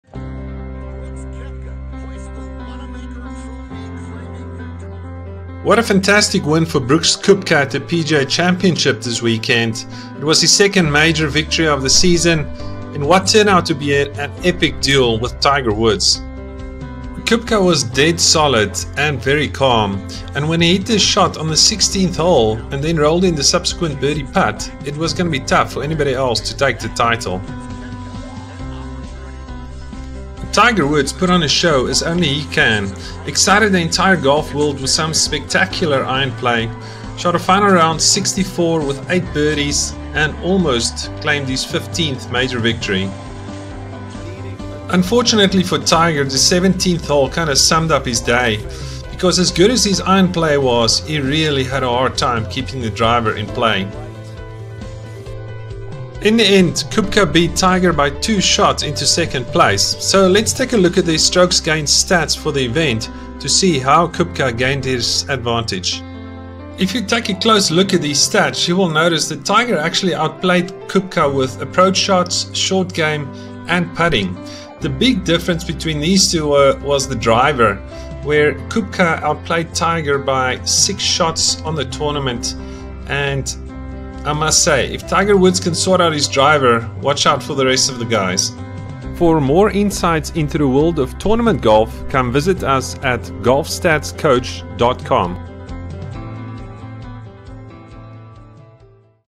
Koepka outduels Tiger to win epic encounter and take the PGA Championship title
What an epic duel between Brooks Koepka and Tiger Woods at the PGA Championship.  Tiger outplayed Koepka in 3 of the four main parts of the game and still lost.  How?